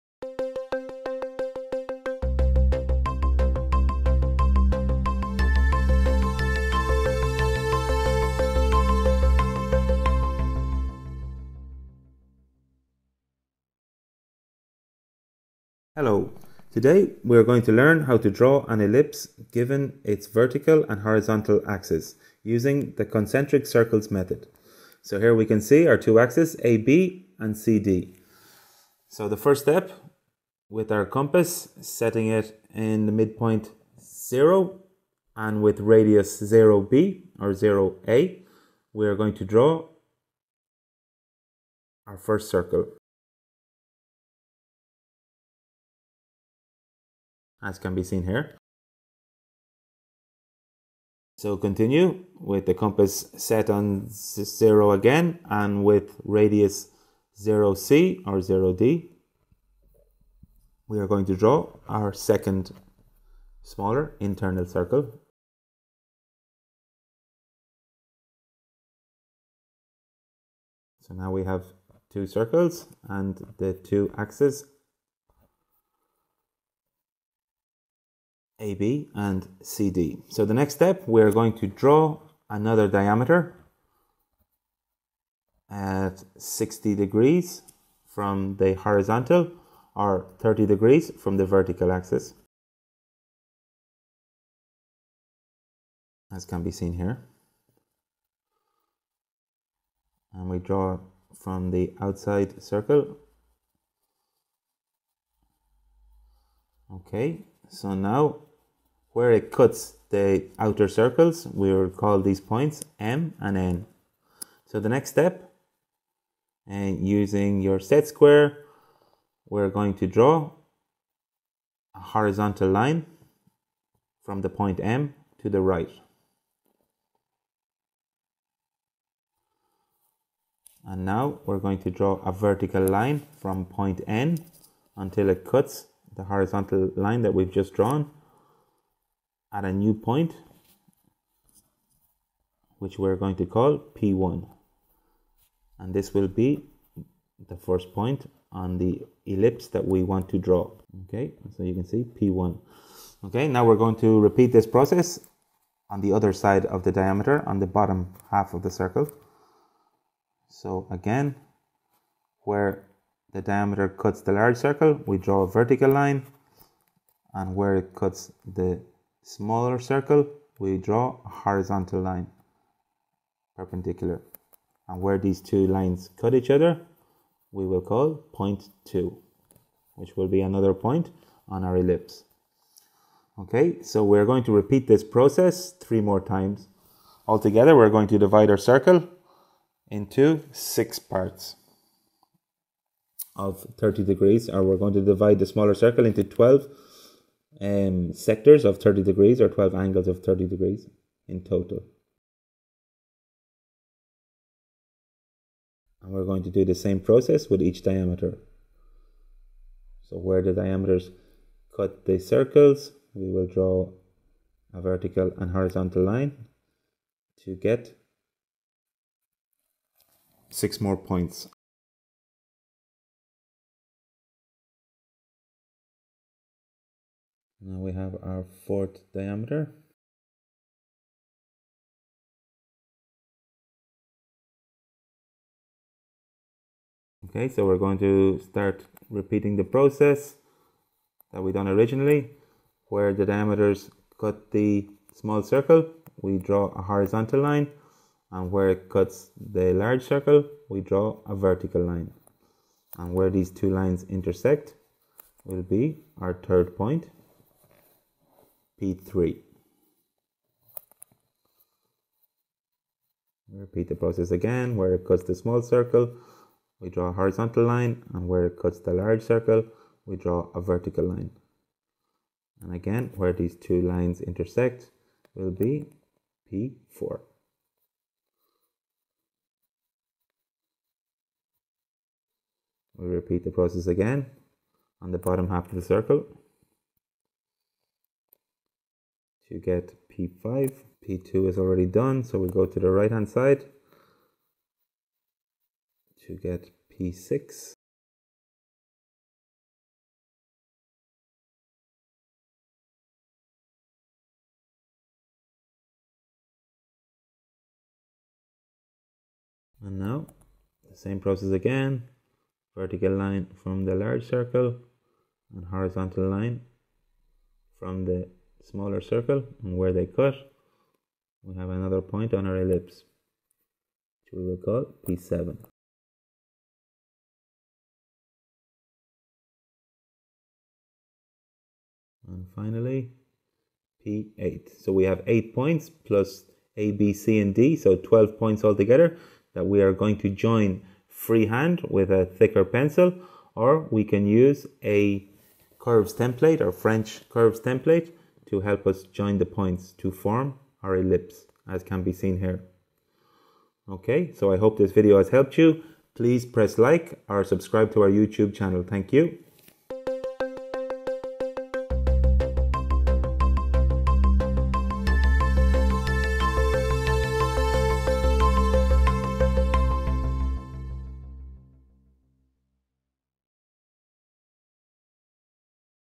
How to draw an ellipse by concentric circle method
Learn how to draw an ellipse given its two axis by concentric circle method.